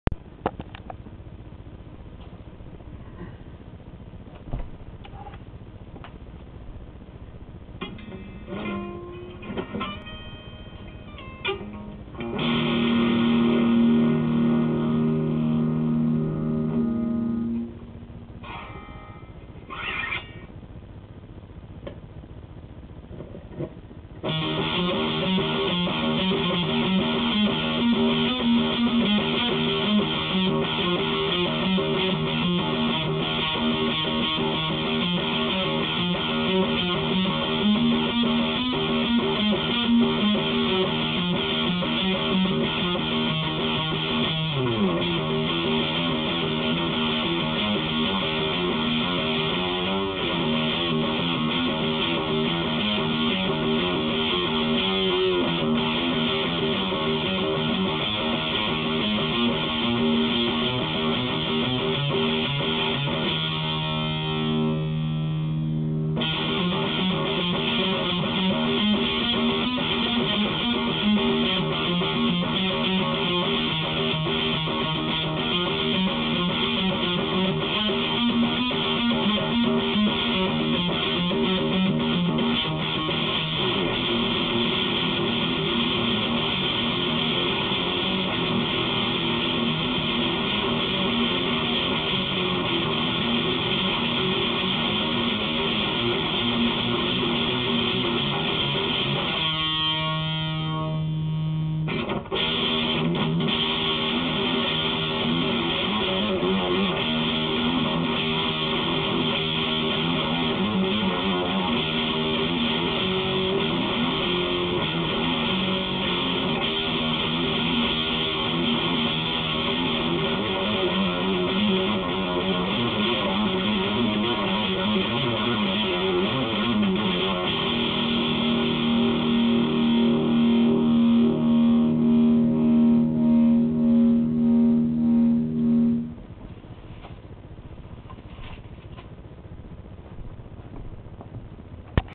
Just me playing
just me fiddle around with a couple licks i made up.